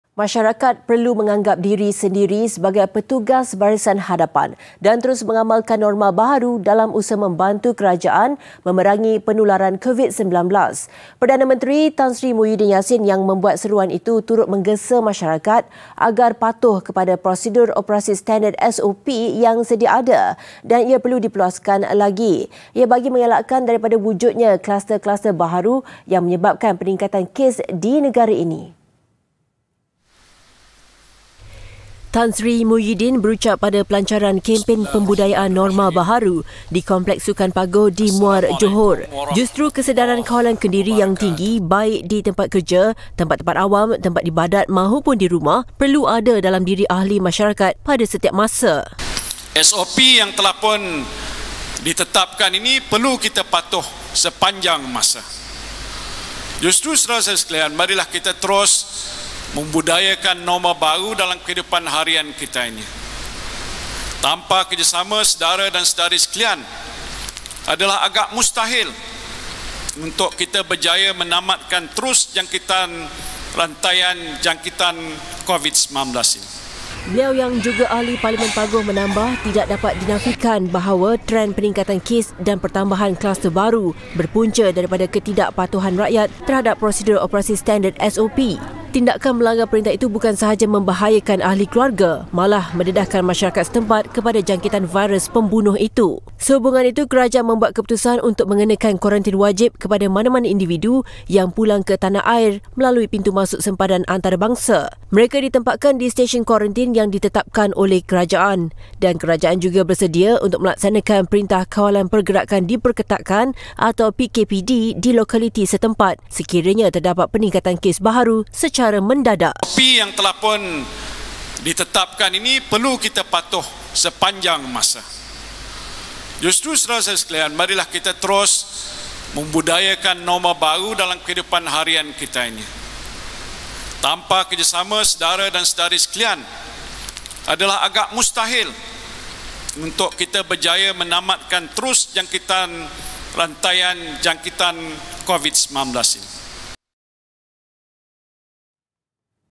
NORMA BAHARU | Anggap Diri Sendiri Petugas Barisan Hadapan
Masyarakat perlu menganggap diri sendiri sebagai "petugas barisan hadapan" dan terus mengamalkan norma baharu dalam usaha membantu kerajaan memerangi penularan COVID-19.